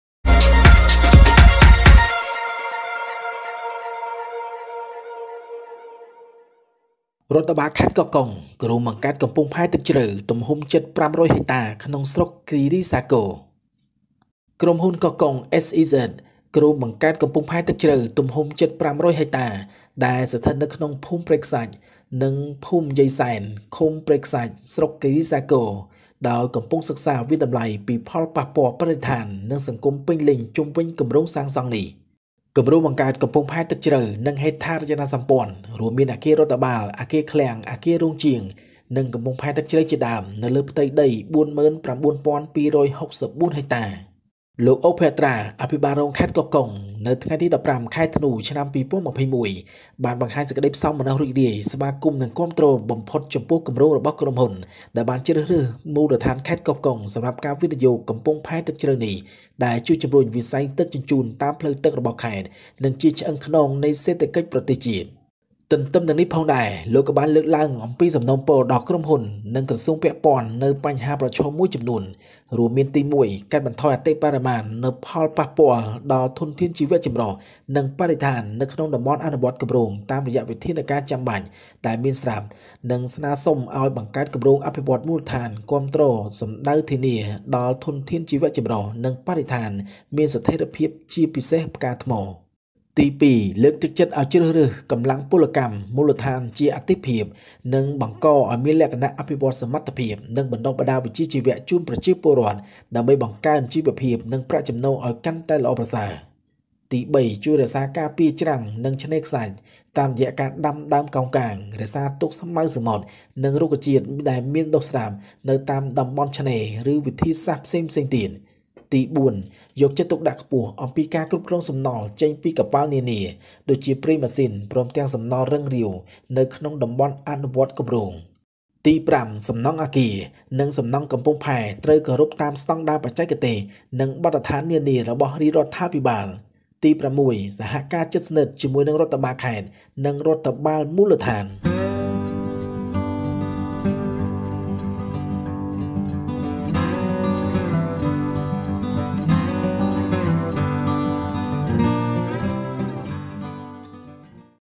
ក្រុមហ៊ុនលីយ៉ុងផាត់ គ្រោងសាងសង់កំពង់ផែទឹកជ្រៅថ្មីមួយកន្លែងលើដីជិត៥០០ហិកតានៅកោះកុង
Koh Kong SEZ Co., Ltd. plans to build a nearly 500-hectare deep-sea port in Prek Khsach and Yeay Sen villages, Prek Khsach commune, Kirisakor district, by conducting a full environmental and social impact assessment on the construction project.  This.
 The project to build a deep-water port and infrastructure, including administrative buildings, warehouses, workshop buildings and deep-sea ports, covers an area of ​​492.64 hectares. Mr. Ouk Pheaktra, Deputy Governor of Koh Kong Province, on December 15, 2021, expressed his pleasure to welcome and support the project of the company that has selected Koh Kong Province for the investment of this deep water port, which will help boost the water transport sector.  Of the province and is the backbone of the national economy. At the same time, he also raised suggestions to companies and ministries related to a number of challenges, including: First, minimize the impact on biodiversity and the environment in the project implementation area through the necessary measures and request the establishment of supporting local development projects to ensure the stability of biodiversity and environment, especially corals. Second, to encourage the selection of local labor as a priority and to develop the capacity and vocational training of the people to increase their livelihoods and incomes. Third, help protect the shores and beaches by planting mangroves, seagrass and plants that grow in coastal areas or other methods.

 Fourth, pay close attention to the management of waste from ships such as motor oil and solid-liquid waste in the project implementation area. Fifth, the construction of buildings and ports must comply with the technical standards and norms of the Royal Government.

 Sixth, close cooperation with the provincial administration and local administration.